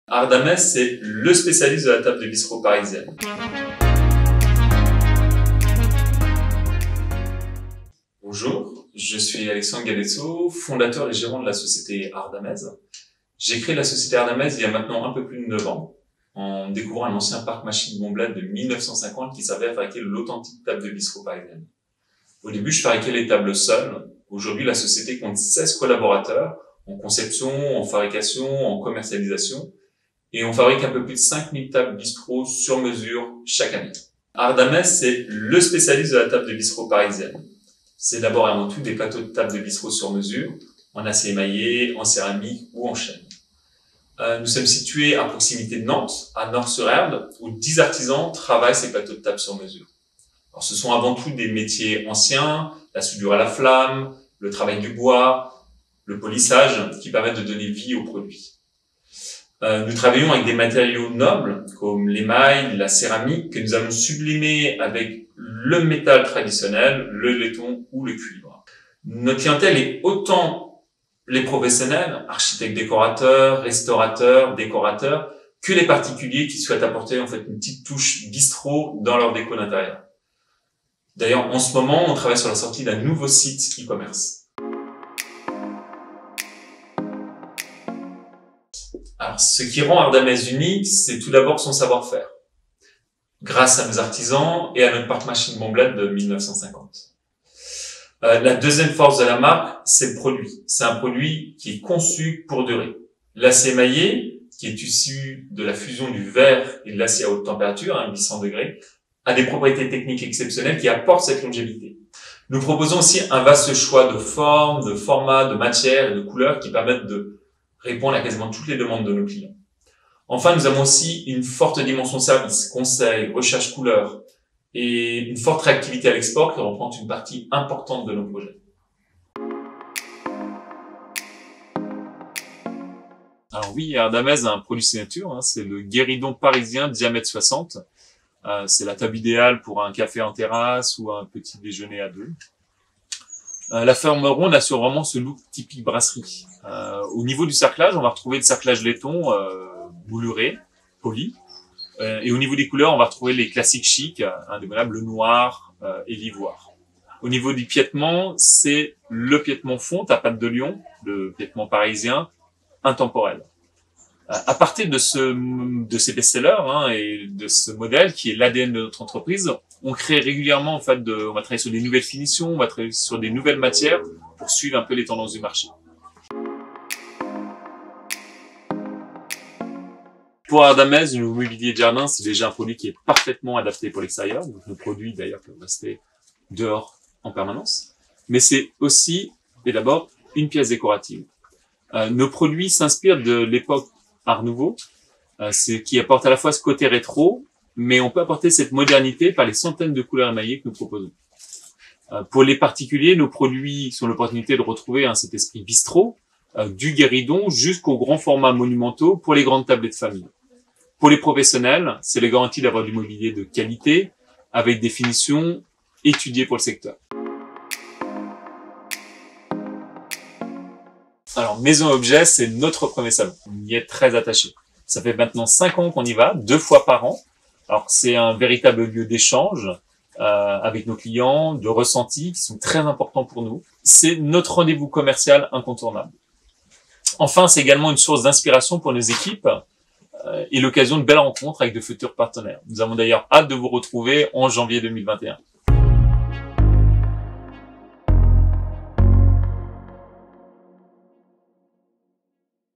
The Face Behind The Brand: ARDAMEZ
What makes Ardamez unique is first of all our expertise, provided by our artisans and our 1950s Bombled machines. Our second strength is our product, which is designed to last.